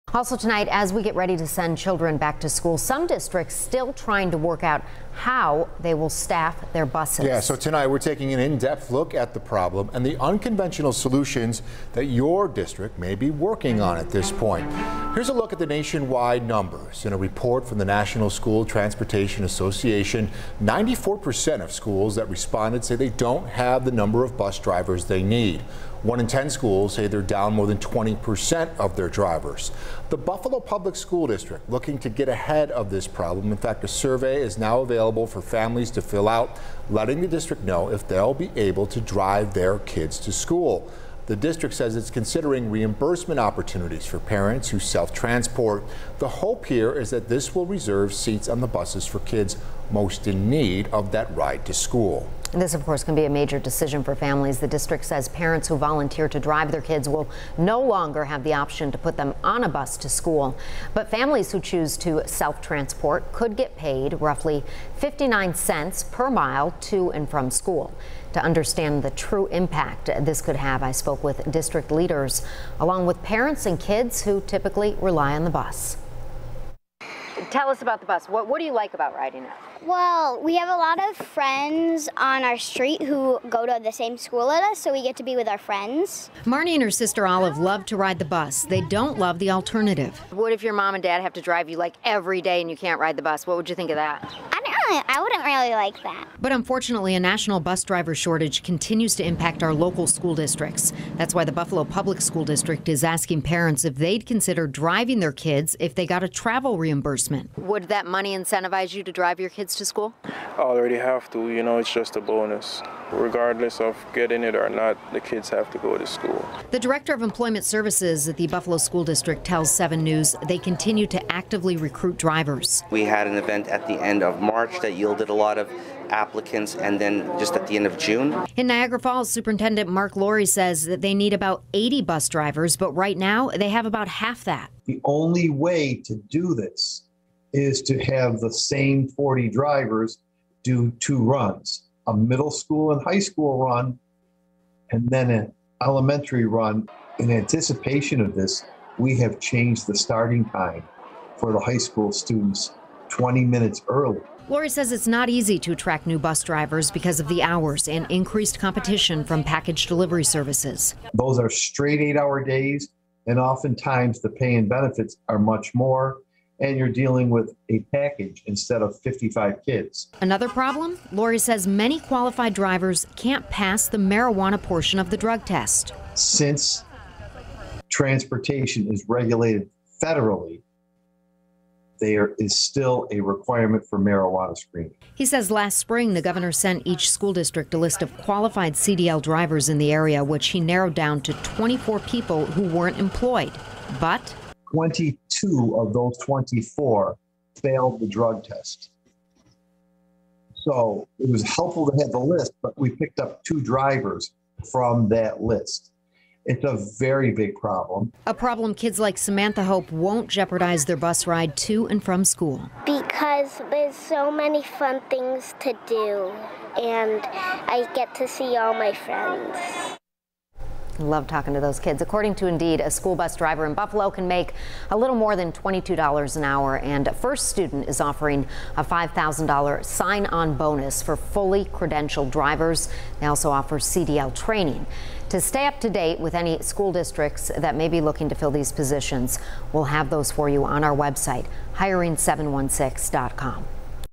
National bus driver shortage predicted to impact Buffalo public schools this year
The Buffalo Public School District anticipates that the national bus driver shortage will impact yellow bus transportation this upcoming school year.